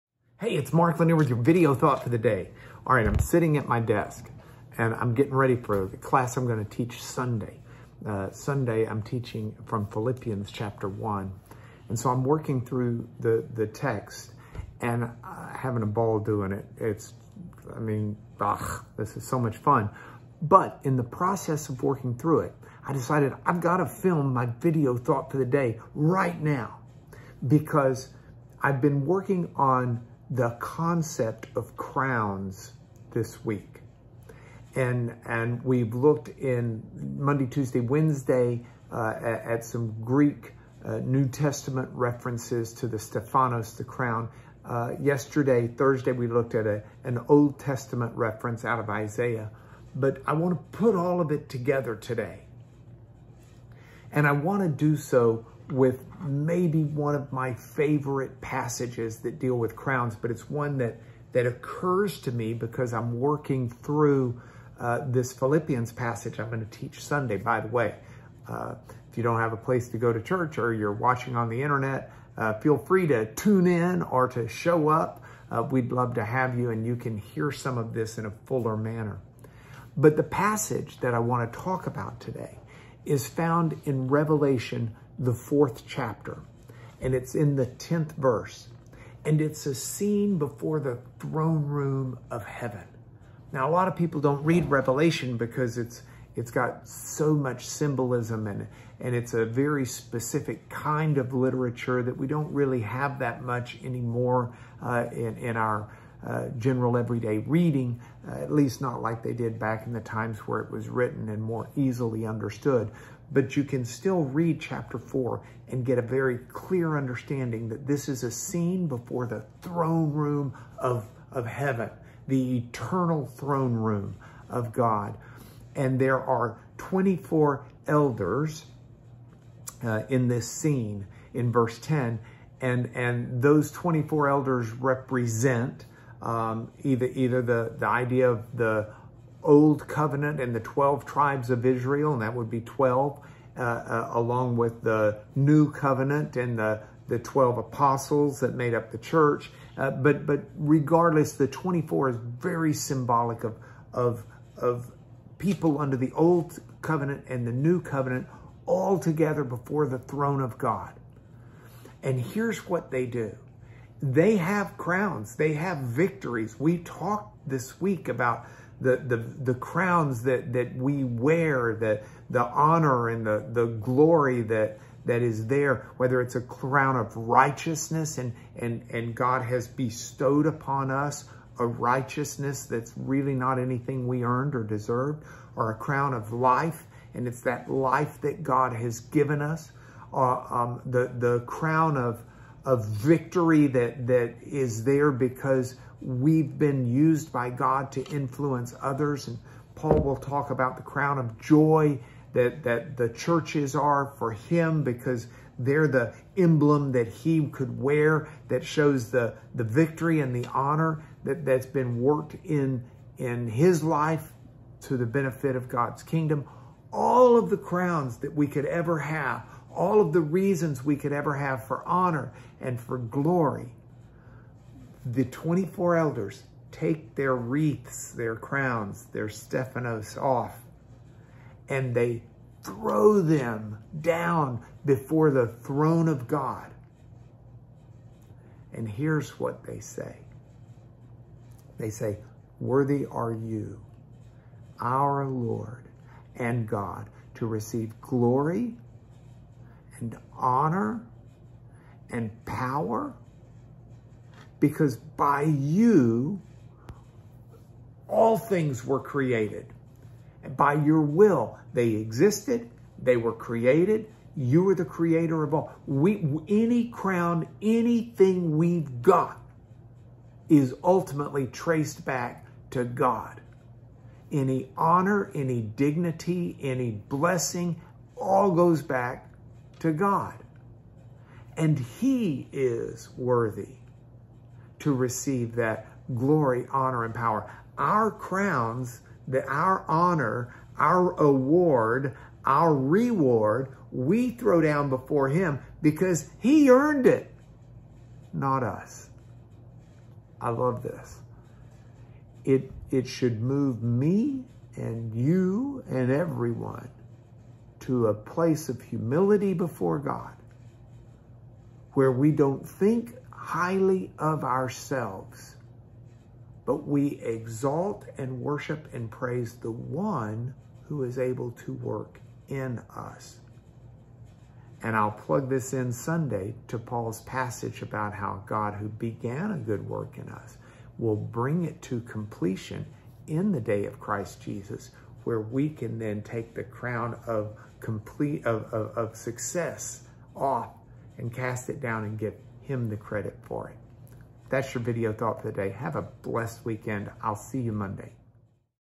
Mark's Thought For The Day - 06/25/21 "What to Do With this Crown?"
Mark takes us to Revelation. The scene he takes us to, is where scripture describes the 24 elders around the throne of God.

It is where those 24 elders who have all of those crowns, for various things in the past, anything that has happened is traced back to God. So they throw all of those crowns before Him.

This is a powerful telling of what happens to our crowns.

Listen!